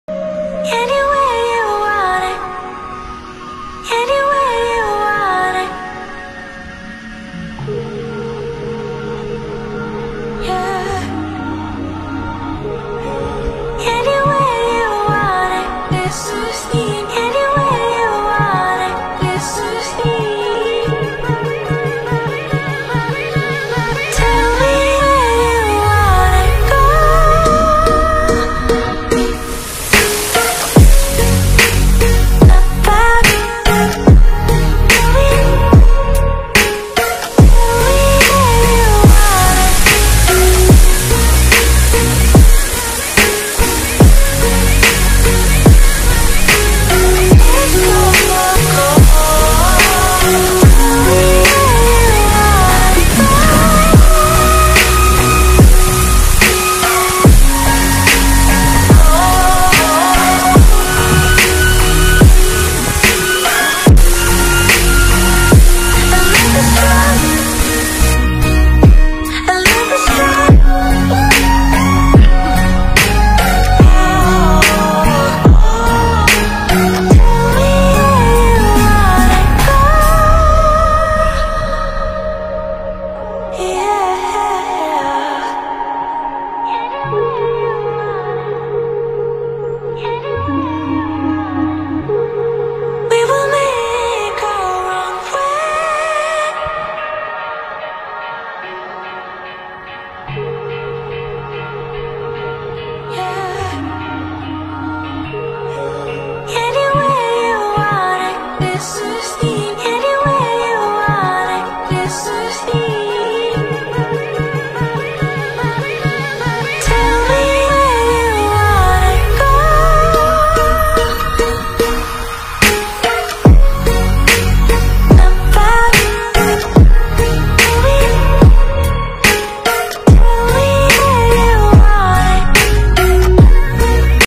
Poção de Muriqui na Costa Verde
Local para relaxar e curtir a natureza em Mangaratiba